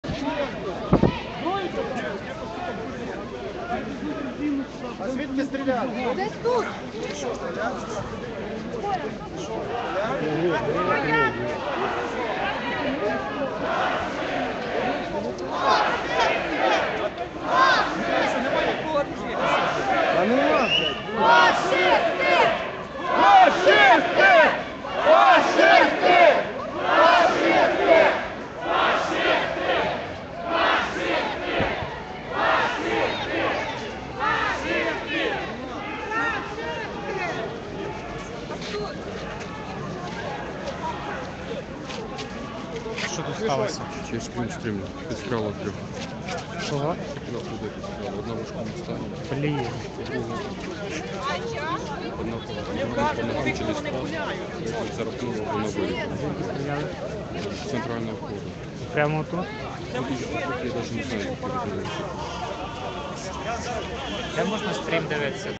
Мітинг біля СБУ. По хмельничанам сбушники відкрили вогонь 19.02.2014
По хмельничанам сбушники відкрили вогонь.